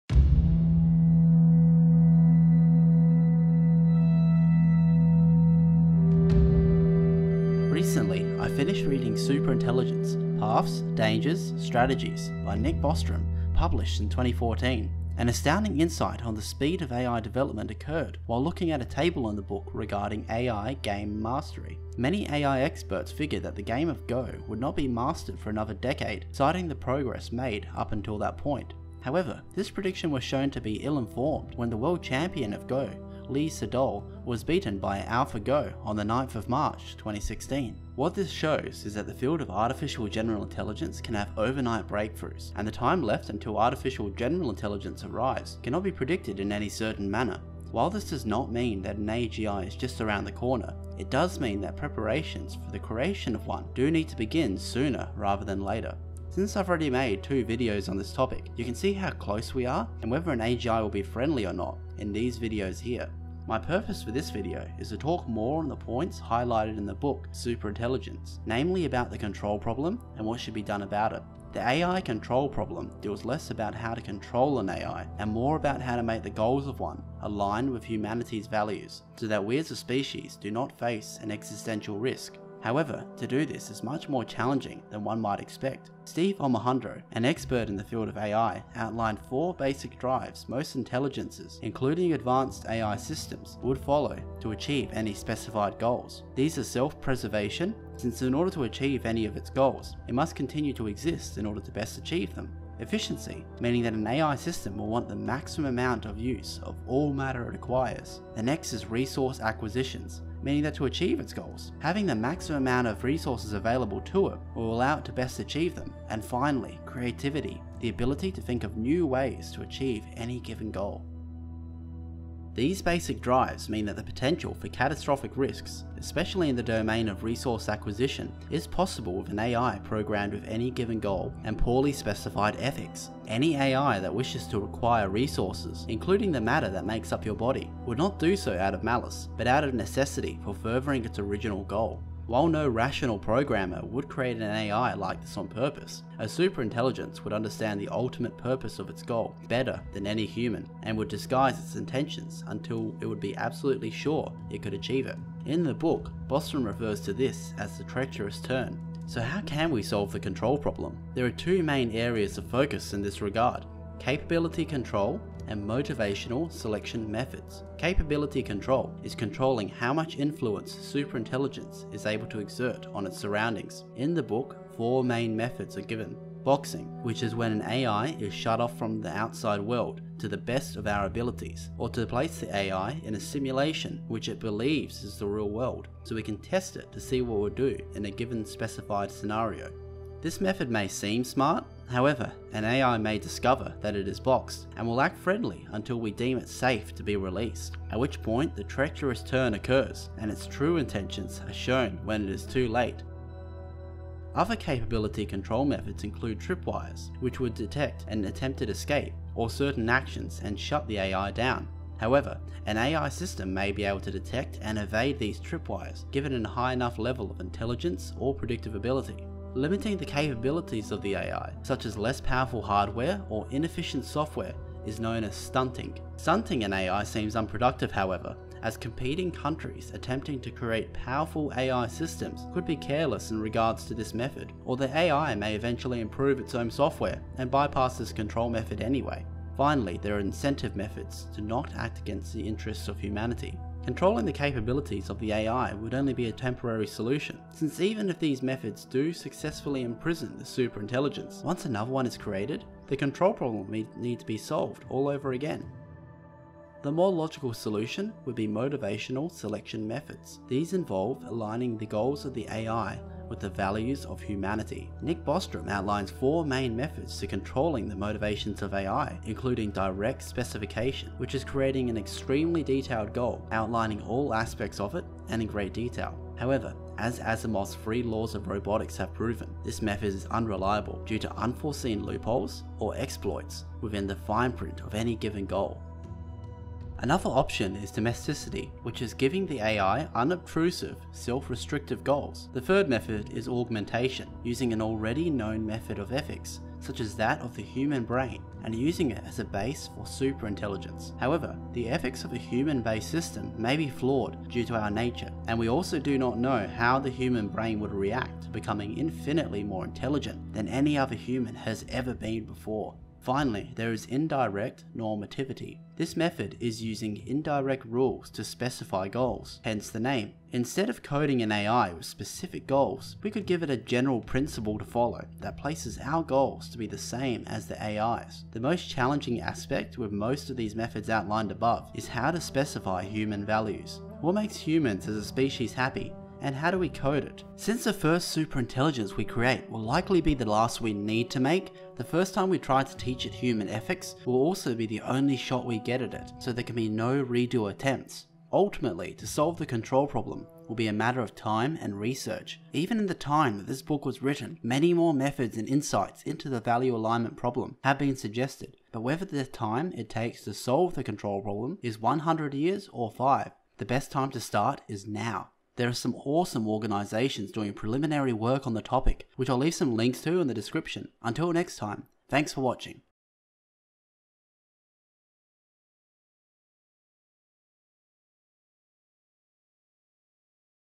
The AI Control Problem
If a superintelligence is created, how would we ensure it is safe and does what is beneficial for mankind?